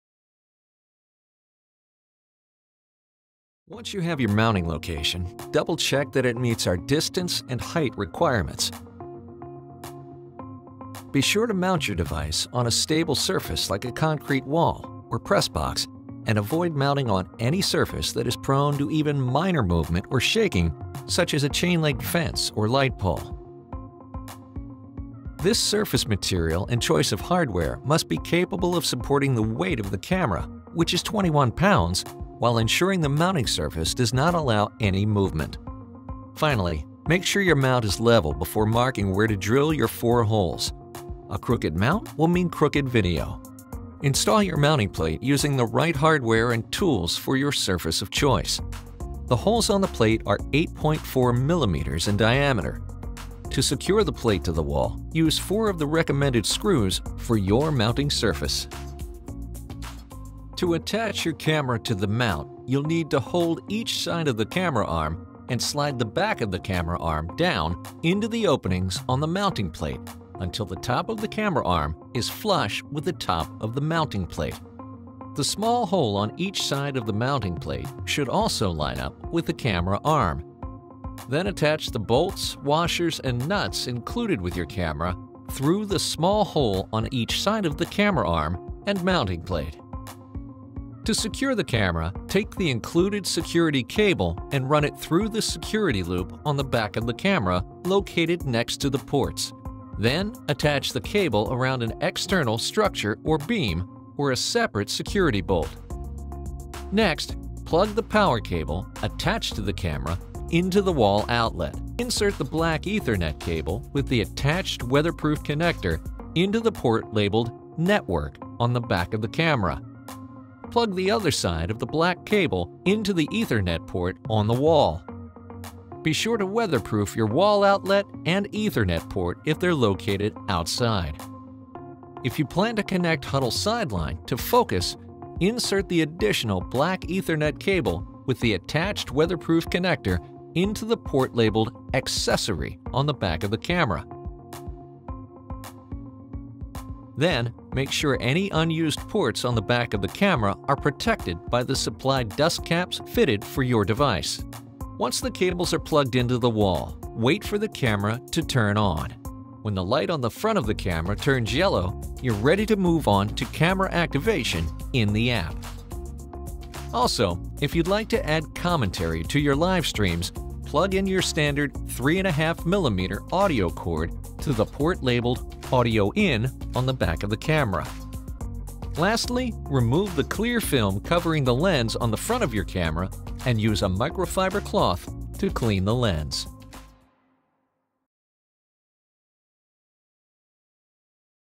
Install and Calibrate Your Camera • Focus Outdoor
Learn how to install your Focus Outdoor camera from physically mounting the camera, to calibrating the camera through the Focus app.

0:00 Mounting Your Camera
1:00 Attach Your Camera
1:30 Connecting Your Camera